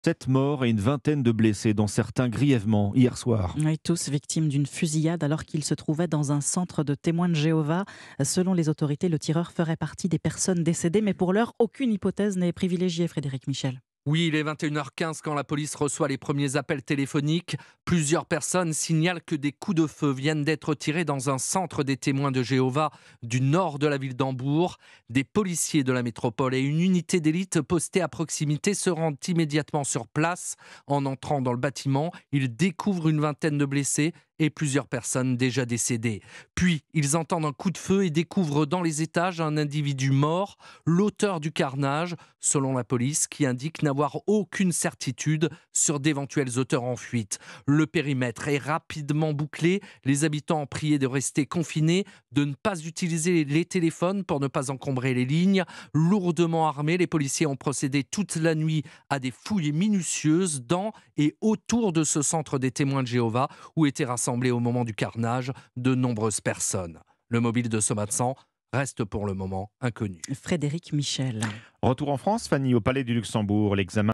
Fusillade à Hambourg : plusieurs morts dans une église des Témoins de Jéhovah, le tireur aurait é…
Fusillade à Hambourg : plusieurs morts dans une église des Témoins de Jéhovah, le tireur aurait été tué